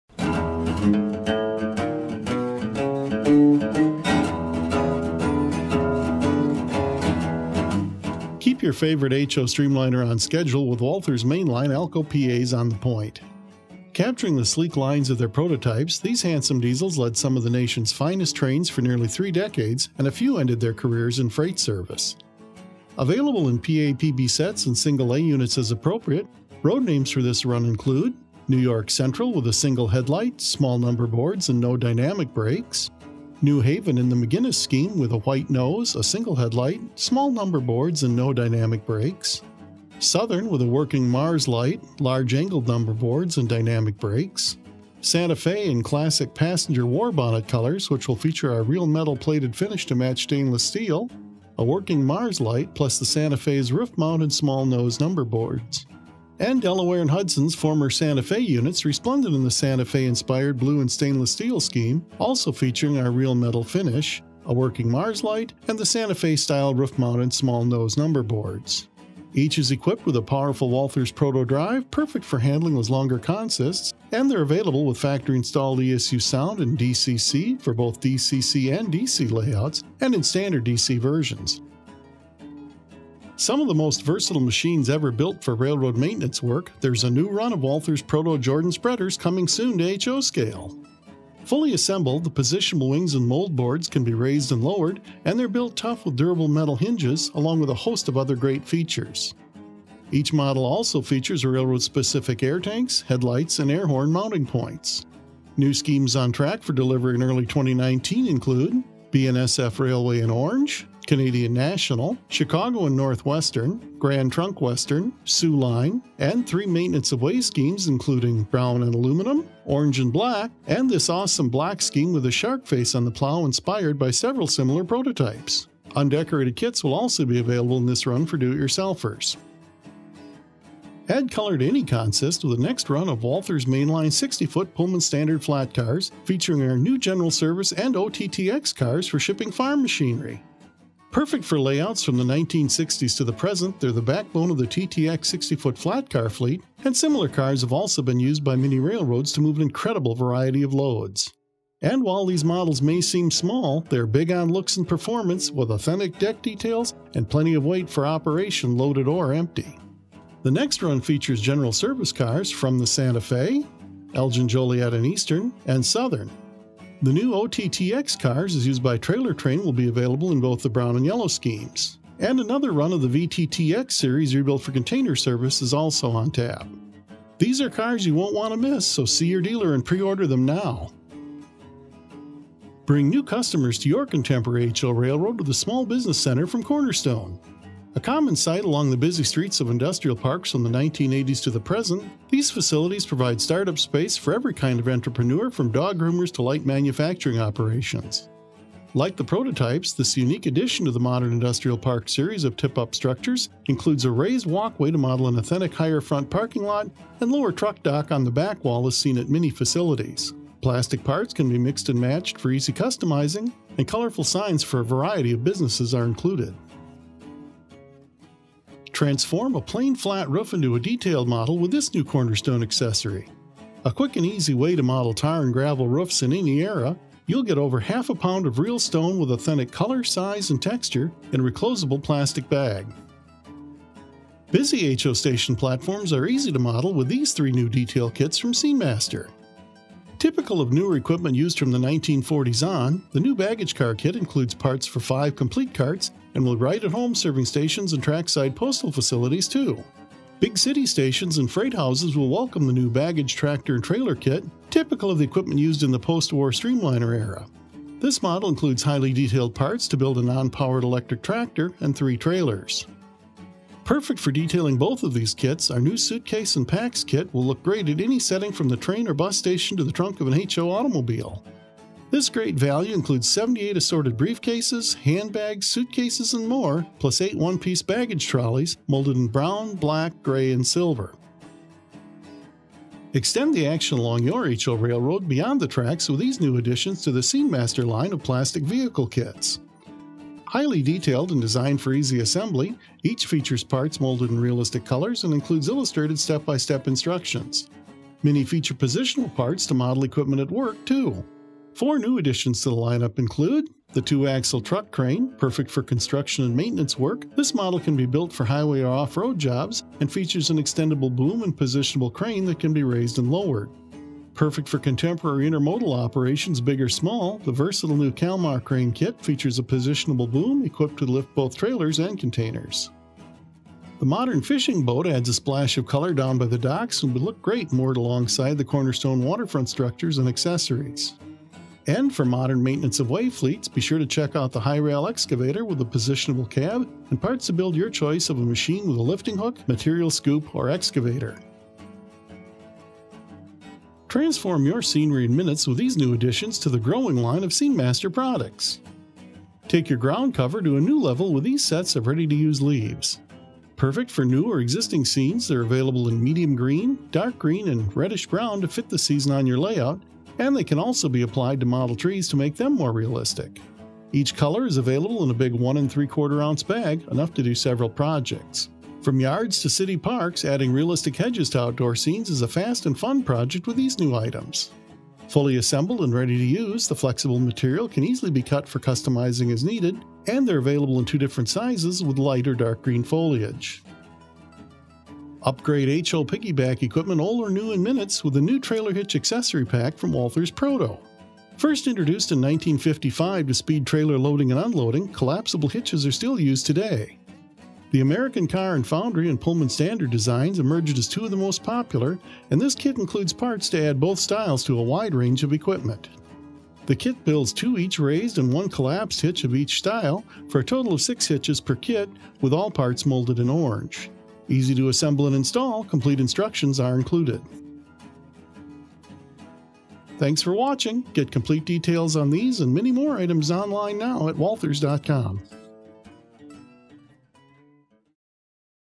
Walthers New Product Express - November 2018
This month’s round of HO Scale announcements showcases new schemes for the super-popular WalthersProto® Jordan Spreader including BNSF, CN, CNW, GTW, SOO as well as three unlettered MOW schemes. New WalthersMainline™ HO Scale Alco PA & PB Diesels featuring ESU® sound and DCC are coming in new schemes as well as additional roadnumbers for roadnames that sold out fast on the first run. Schemes include D&H, NYC, ATSF, SOU and NH. New schemes and a new OTTX body variation are also on the way for WalthersMainline 60' Pullman-Standard Flatcars. The new Cornerstone® Small Business Center brings a typical modern structure to trackside scenes and the Roof Texture Kit makes it easy to add a realistic crushed stone texture to structure rooftops. New SceneMaster™ Baggage, Baggage Carts and Baggage Tractor kits make station platform detailing easy and new construction, railroad and marine vehicles as well as leaves and hedges make adding realism to your layout easy.  See all of the new announcements in this video!